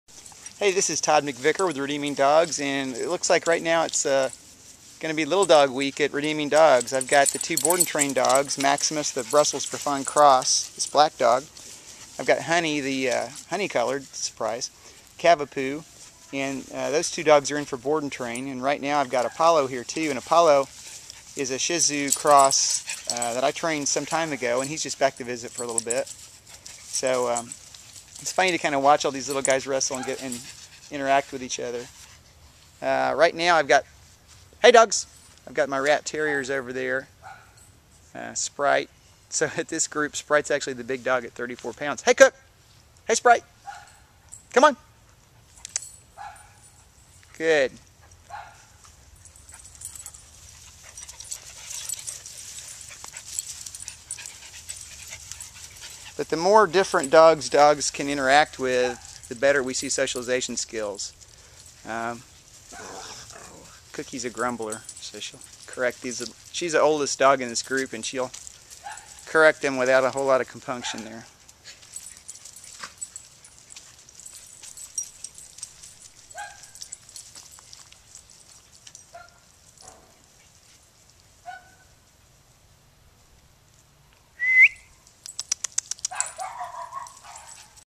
Small dog socialization | Redeeming Dogs | Dallas Dog Training
I've got Honey the cavapoo, Maximus the brussels griffon cross, and Apollo the Shih Tzu cross - of cours my dogs Cookie and Sprite are here too. Redeeming Dogs trains dogs and owners in the Dallas/Fort Worth area.